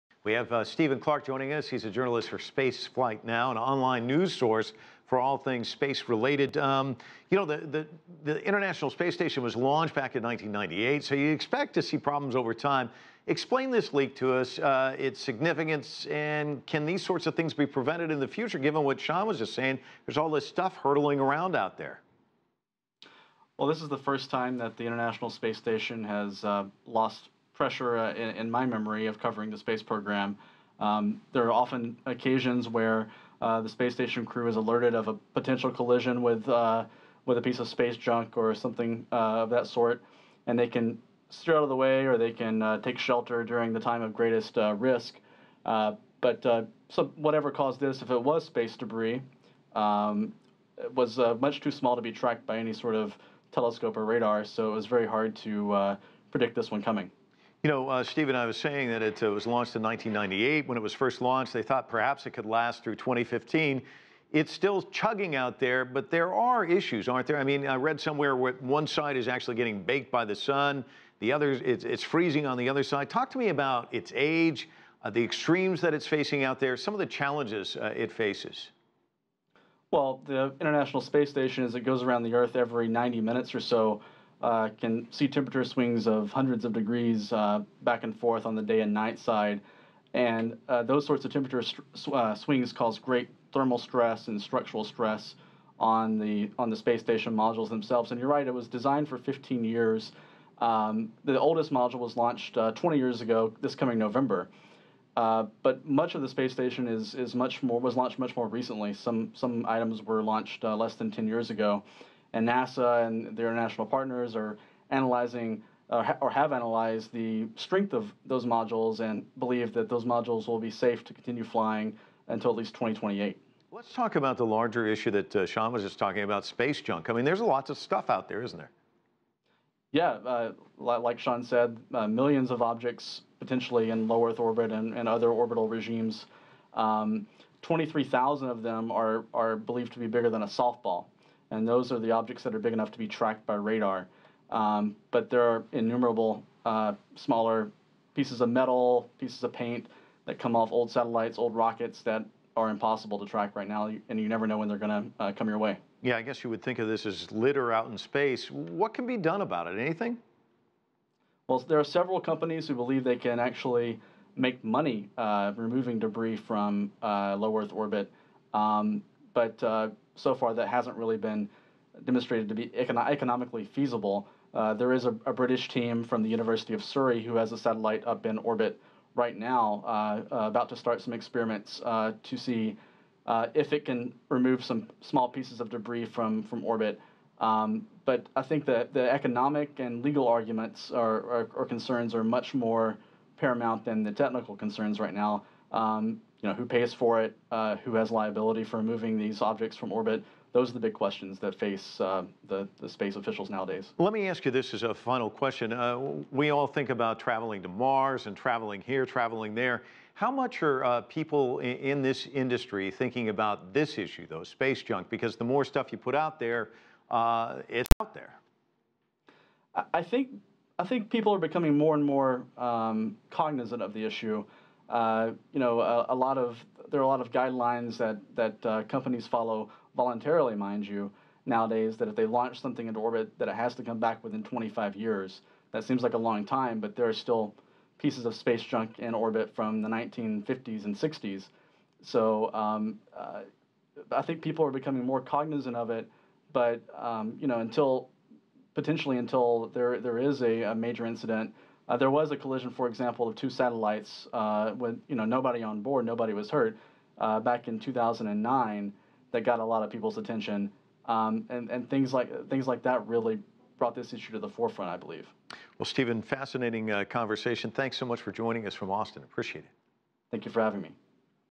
Stephen Clark of Spaceflight Now discusses the air leak on the ISS
CGTN's Mike Walter talks to Stephen Clark of Spaceflight Now about the reported air leak on the International Space Station.